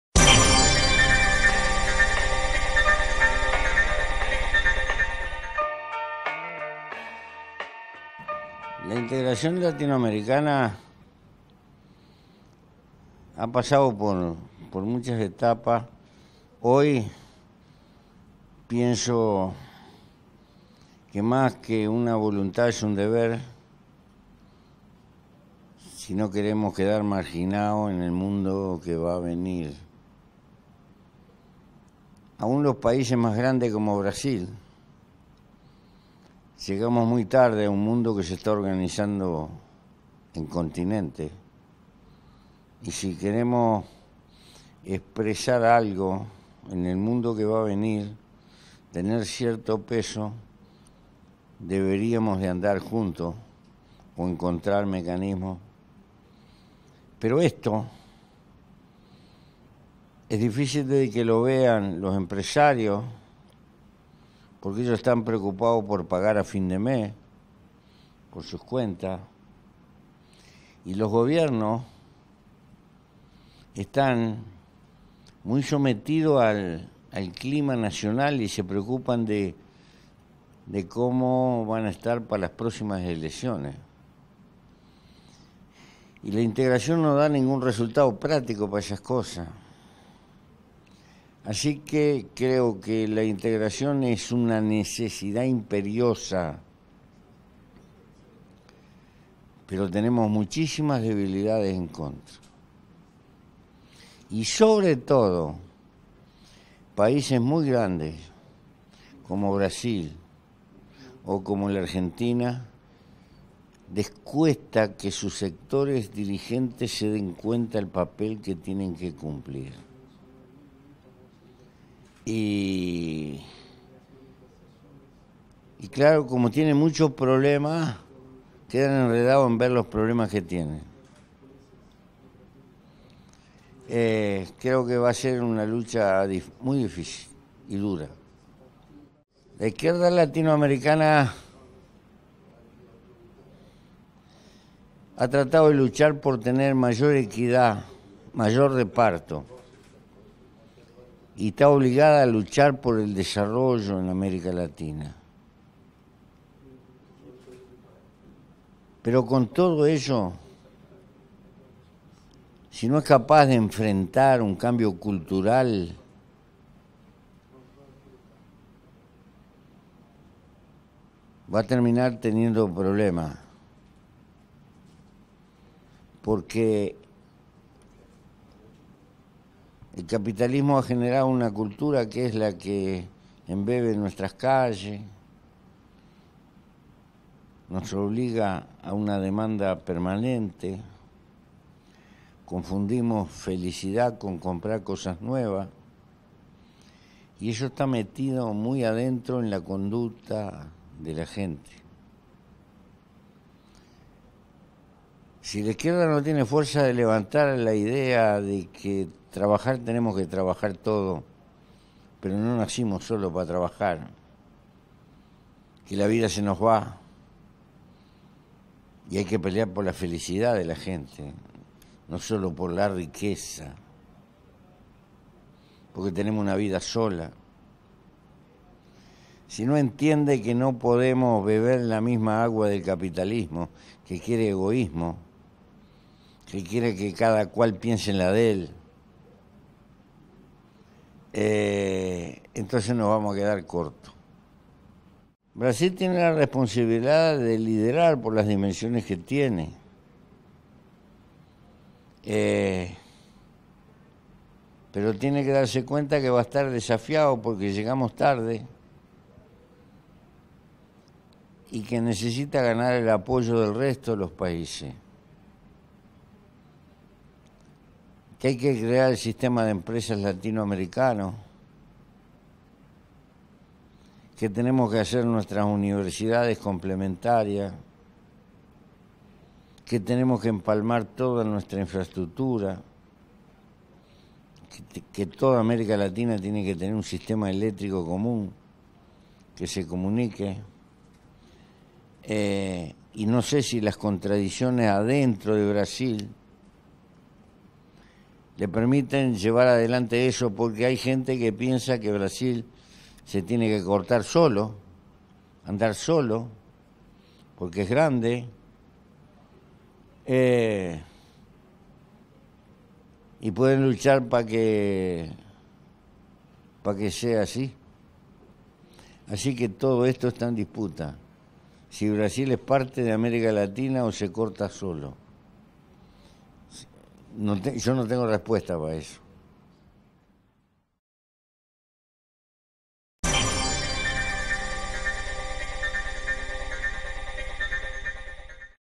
Pepe Mujica: a integração latino-americana e o papel do Brasil
Entrevista exclusiva com Pepe Mujica, senador e ex-presidente do Uruguai.